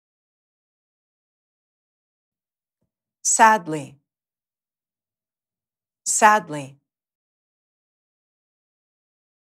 How to pronounce SADLY in American English
This video shows you how to pronounce SADLY in American English. Speaker has an accent from Winston-Salem, North Carolina. Collins Dictionary, the home of living English and pioneers of dictionary publishing